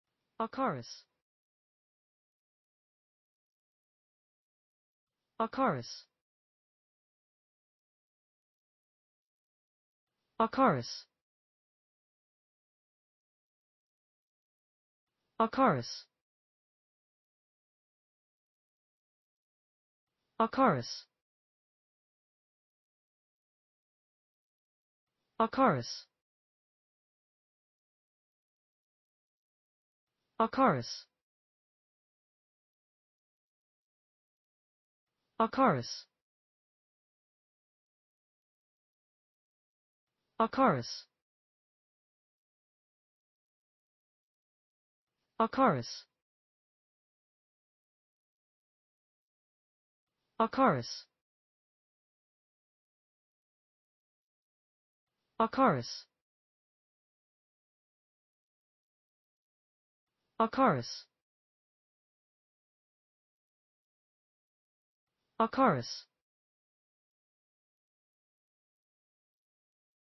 acarus: How to pronounce acarus in english (correct!).Start with A. Learn from me.
Follow me!Learn from me how to pronounce the word acarus in english.This video shows you how to pronounce the word acarus in english.

acarus

n. 螨；恙虫；壁虱

/ˈækərəs/

1.   N   名词

any of the free-living mites of the widely distributed genus Acarus, several of which, esp A. siro, are serious pests of stored flour, grain, etc (生在粮食中的有害)螨虫